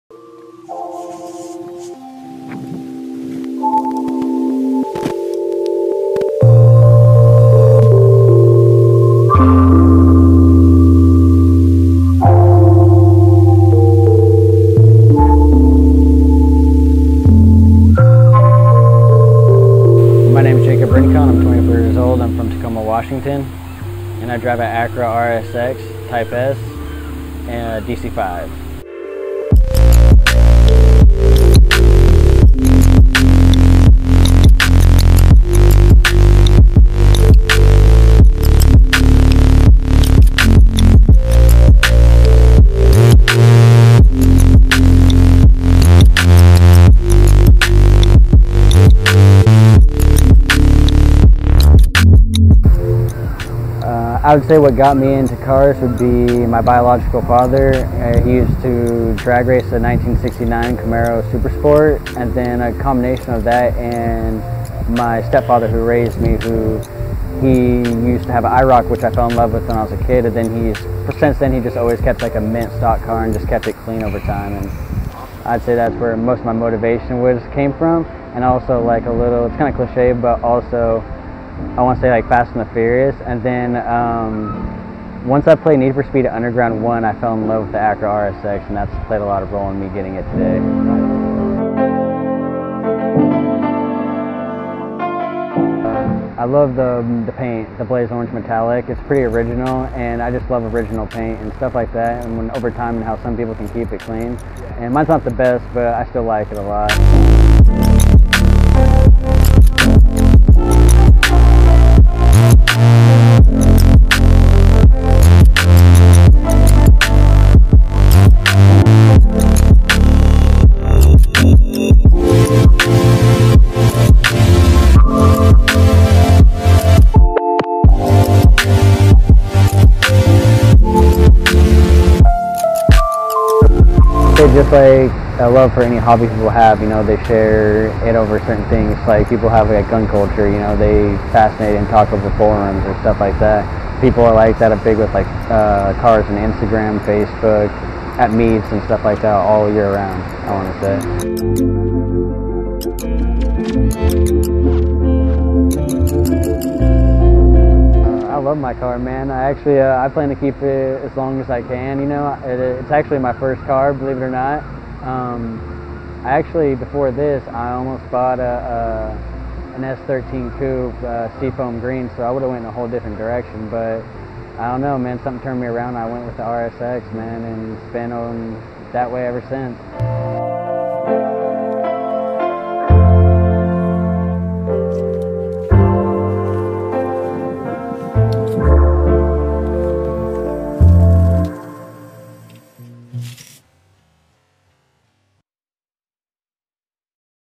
Chariot Ep.4| Jacob's DC5
Chariot goes beyond the standard of most current automotive features. This series aims to showcase not just the car, but the person behind the build, and their thoughts, visions, struggles, and unique style that makes their build, their work of art, stand out from the crowd. This episode showcases Jacob Rincon, and his Acura Integra DC5. Learn the history of his build and his unique experiences and opinions about the car scene. Tune in August 15th for the next installment in the Chariot series!